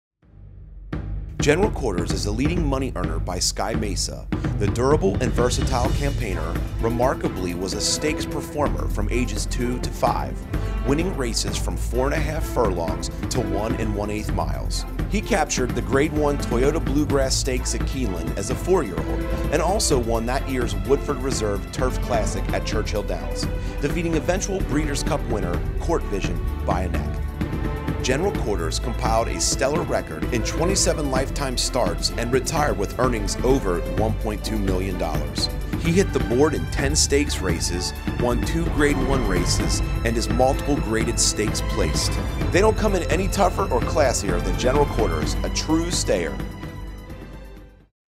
General Quarters
2006 Gray or Roan Stallion
Sky Mesa x Ecology (Unbridled's Song)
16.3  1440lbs
Woodford Stallions LLC standing at Stonewall Farm Ocala